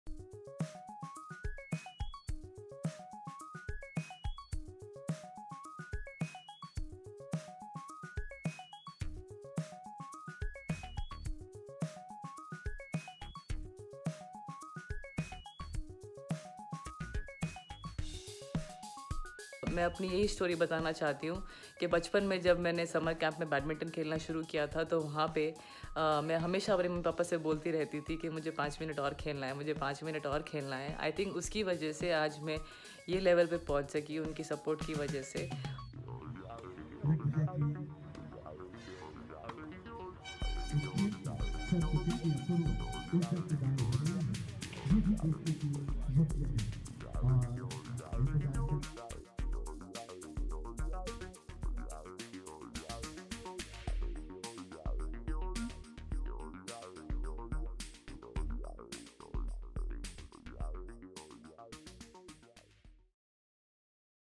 Salman Khan, Saina Nehwal respond to Rajyavardhan Rathore’s 5MinuteAur challenge
Rajyavardhan Singh Rathore launched 5MinuteAur challenge asking people to share their own ‘five minutes more’ stories and videos using 5MinuteAur and KheloIndia. He tweeted a video playing table tennis with both hands conveying the message for the challenge. Twitter is all praises for his 5MinuteAur challenge. Salman Khan accepted the challenge by posting a video in twitter. Saina Nehwal posted a video sharing her 5MinuteAur story. Minister of State for Civil Aviation Jayant Sinha posted a video of him playing tennis. Minister of State for Home Kiren Rijiju also shared a video of him playing football.